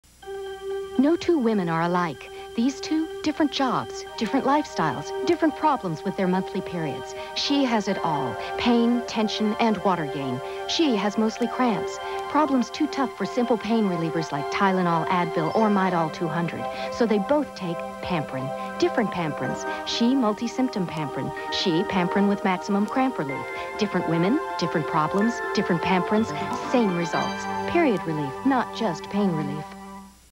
Pamprin ad shown in 1989
"Different women, different problems, different Pamprins ..."
So there were ~2,618,720,779 different kinds of Pamprin in 1989?